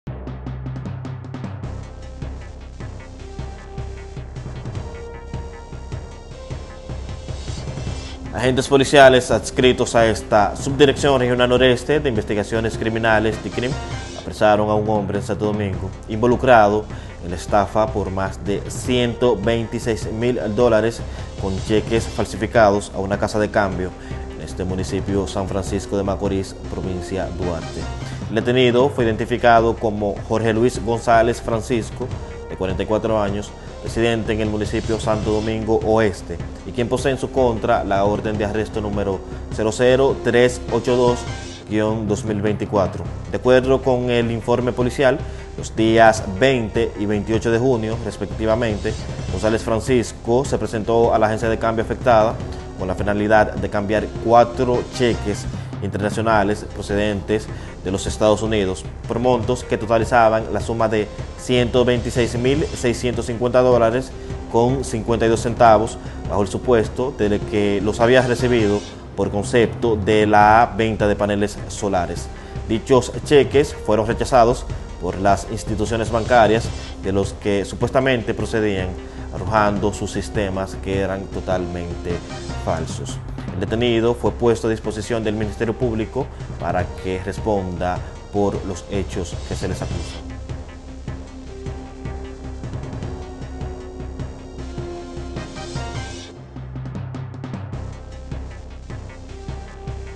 PN apresa a un hombre por estafa con cheques falsos en SFM
San Francisco De Macorís. - Apresaron a un hombre involucrado en la estafa de más de US $126,000 con cheques falsificados a una casa de cambio, según informó la Policía Nacional en la mañana del martes 27 de agosto.

Según informó el vocero del Comando Regional Noreste Duarjeris Palma, el detenido fue identificado como Jorge Luis González Francisco, de 44 años, residente en el municipio Santo Domingo Oeste, y quien posee en su contra la orden de arresto no. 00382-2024.

De acuerdo al informe policial, el pasado 20 y 28 de junio, respectivamente, González Francisco, se presentó a la agencia de cambio afectada, con la finalidad de cambiar 4 cheques internacionales, procedentes de los Estados Unidos de Norteamérica, por monto que totalizan la suma de $126,650.52 dólares, bajo el supuesto de que los habría recibido por concepto de ventas de paneles solares.

Dichos cheques fueron rechazados por las instituciones bancarias de los que supuestamente procedían, arrojando sus sistemas que eran totalmente falsos.

Palma informó que el arresto se realizó por agentes policiales adscritos a la Subdirección Regional Noreste de Investigaciones Criminales (DICRIM)

El detenido fue puesto a disposición del Ministerio Público, para ser procesado ante la justicia.